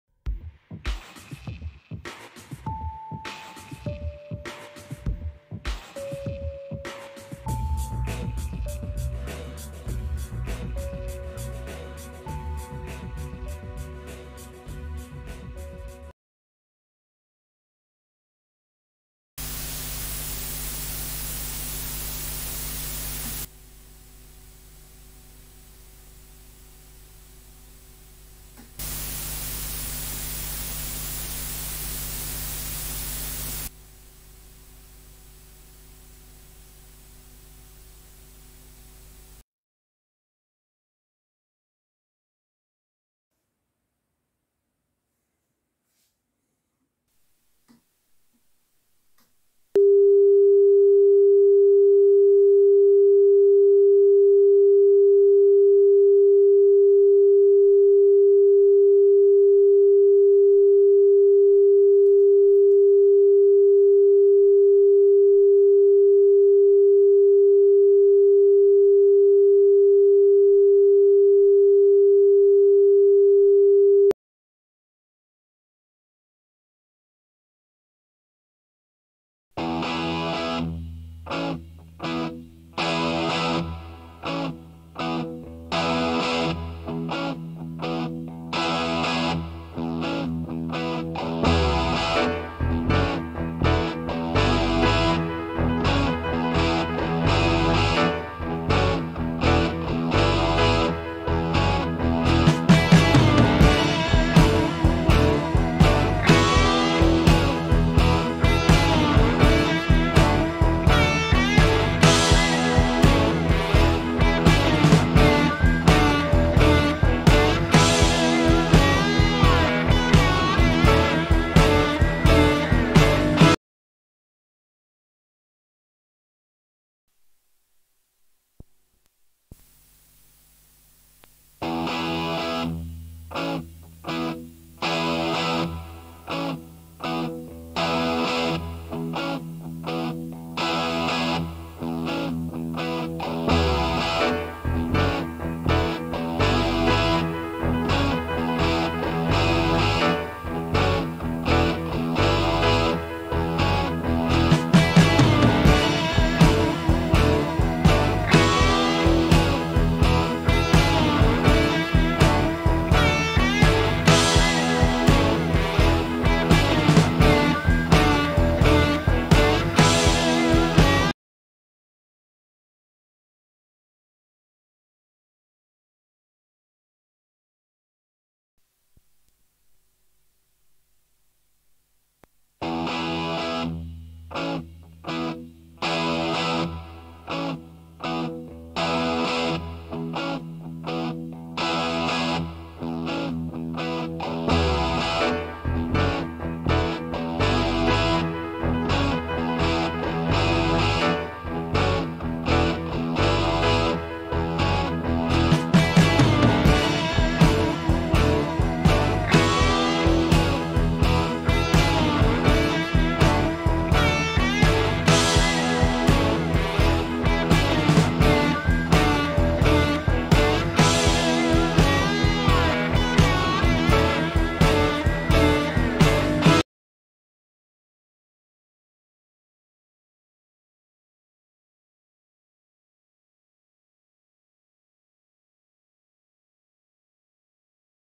Nakamichi HighCom II (1979) noise reduction - Demonstration
This is a Nakamichi HighCom II noise reduction unit from 1979. The unit has been fully recapped and recalibrated.